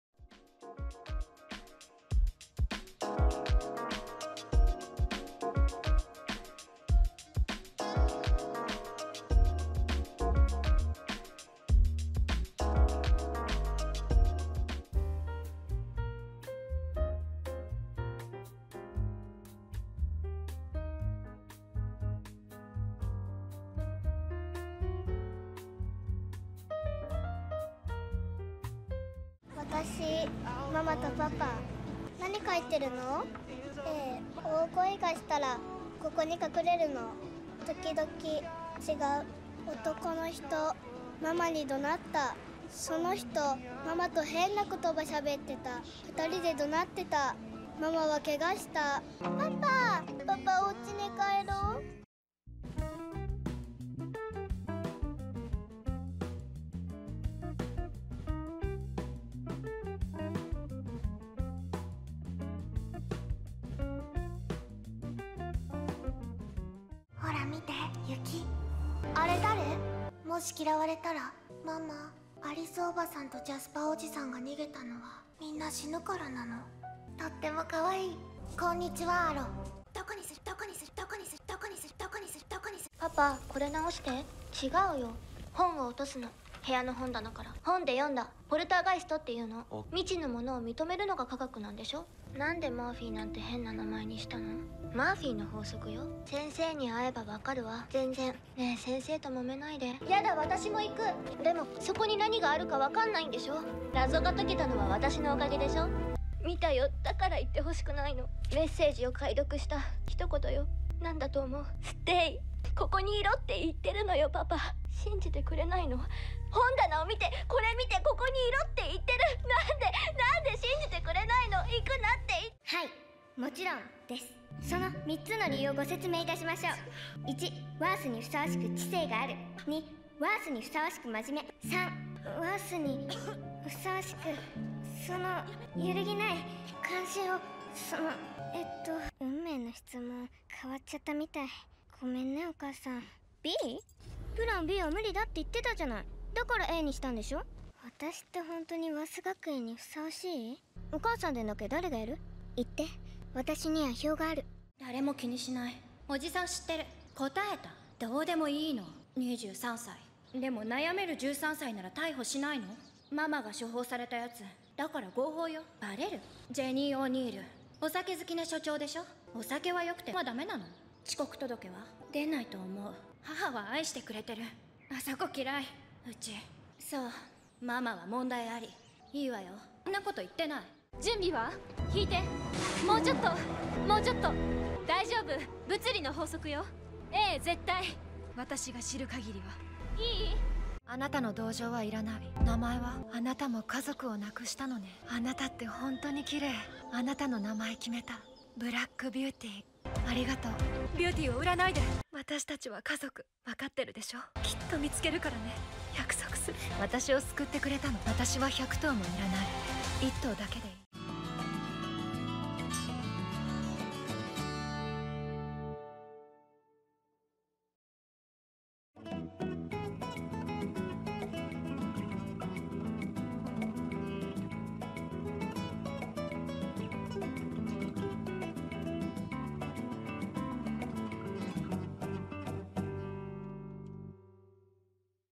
俳優別吹替声優#1056 マッケンジー・フォイ 編
俳優別の吹き替え声優 第1056弾は マッケンジー・フォイ 編です
ソフト版 (BD・DVD・VHS) や配信版・TV版の吹替は誰がやってたのかなぁ???
是非御覧くださいませ、意外な発見がありますよ!!

★マッケンジー・フォイの吹替を担当した声優さん達
潘めぐみ,諸星すみれ,鈴木梨央, 小芝風花,豊崎愛生,
渡辺広子,早見沙織,